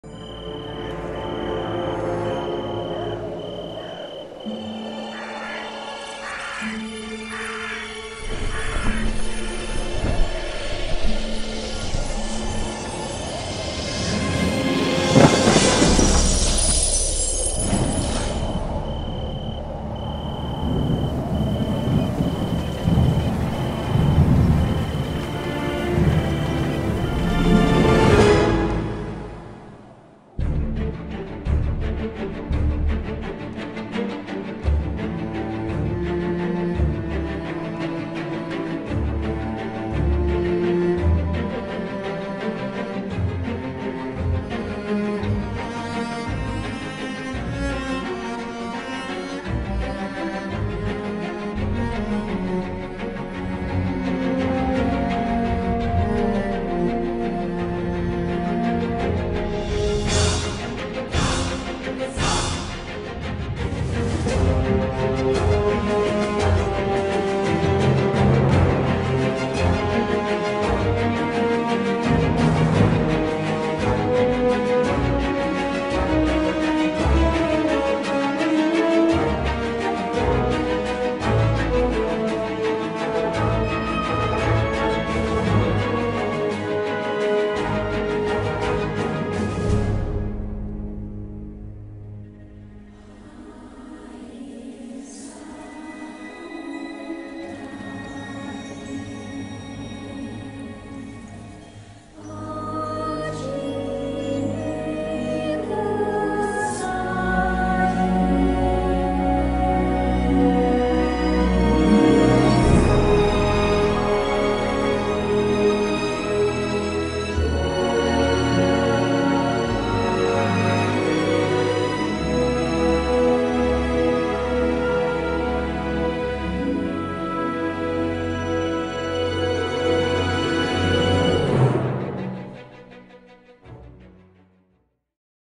GARGOYLE a Matte Painting Breakdown by Maxime Tiberghien
Un matte painting réalisé dans le cadre d'un devoir pour l'institut INGEMEDIA.
A matte painting created for the institut INGEMEDIA.